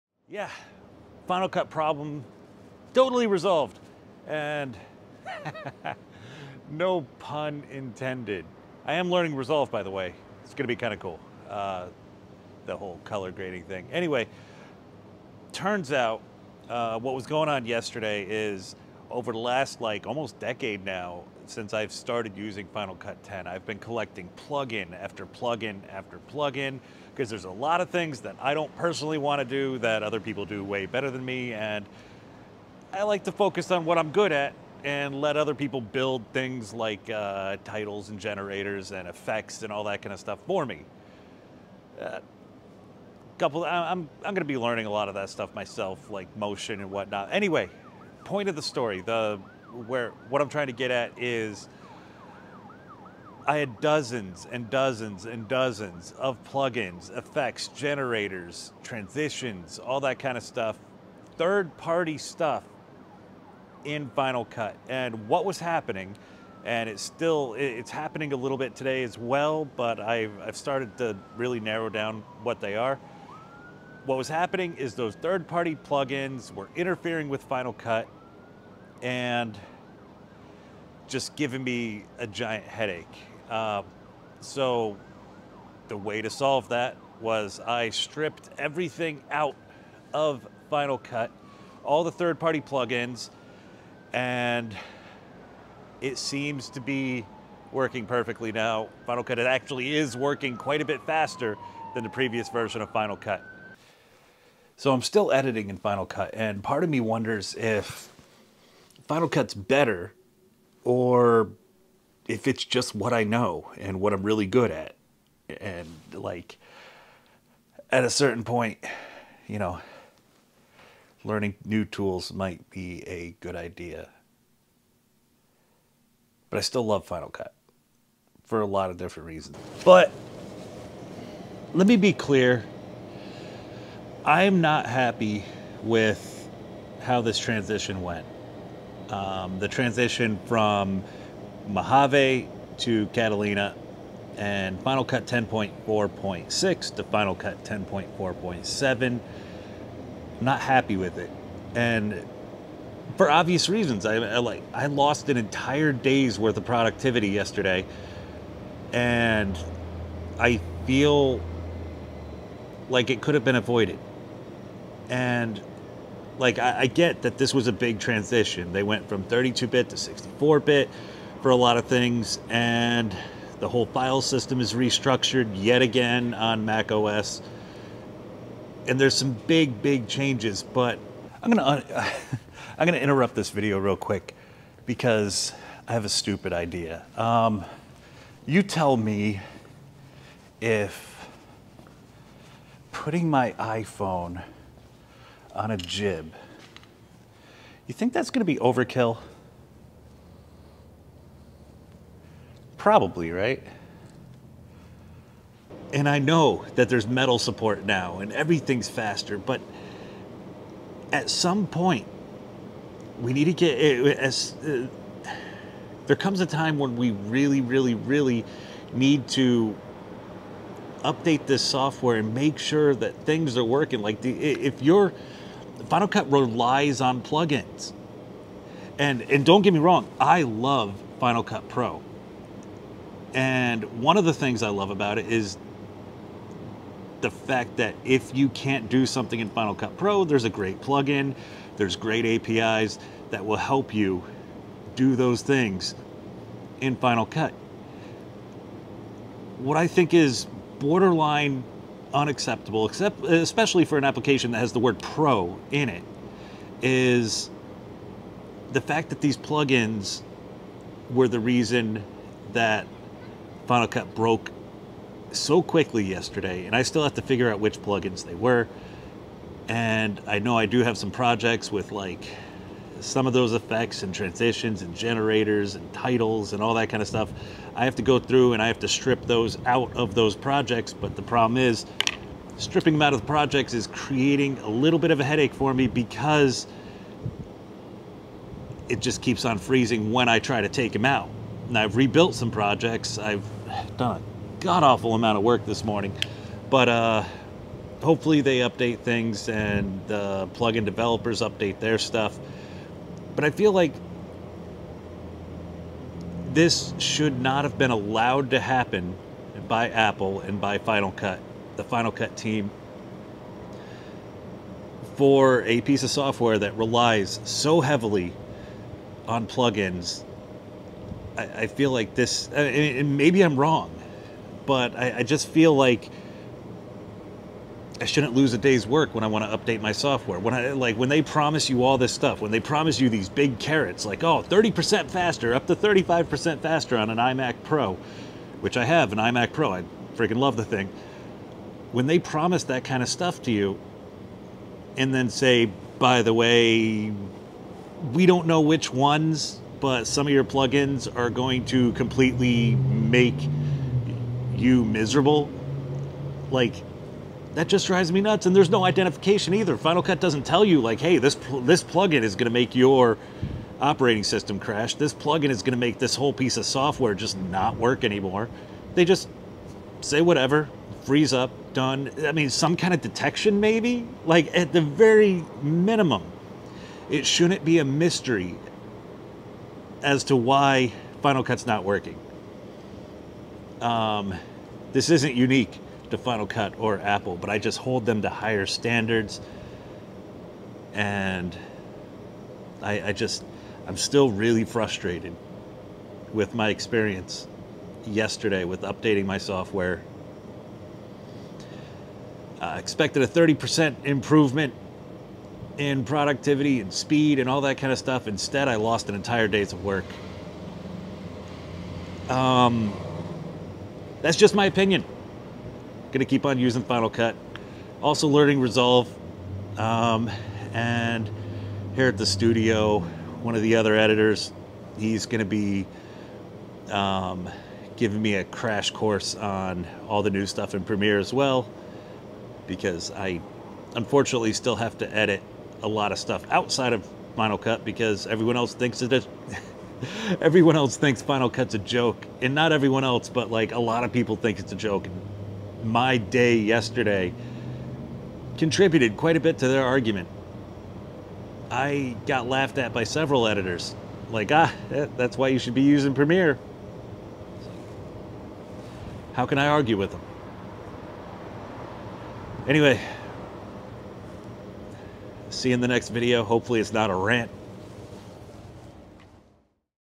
Go Home Final Cut Pro, You're Drunk | 10.4.7 + Mac OS Catalina
Still upset from losing a day of video editing work so bear with me here.

Final Cut Pro X aka FCPX was just updated. It's fast, very fast. Also... it's buggy. One of the core strengths of FCPX is it's massive community of 3rd party plugins, a community that gets seemingly snubbed by Apple on every update. SOMETHING has got to give here and someone on the final cut team should have anticipated this insanity with plugins, at the VERY LEAST by making a way to detect which plugins are causing problems.